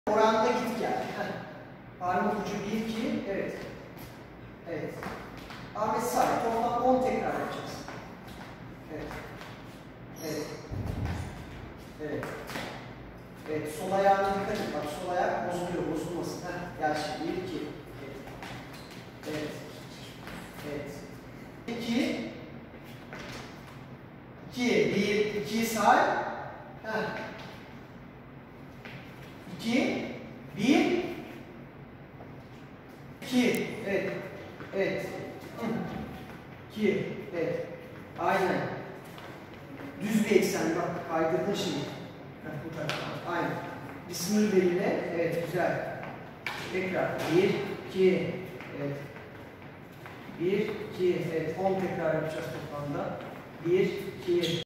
Table Tennis Side Step Exercise with Resistance Band
Direnç bandı ile Masa Tenisi yana kayma adımı çalışması.  ( Ahmet, Eren ve Manas )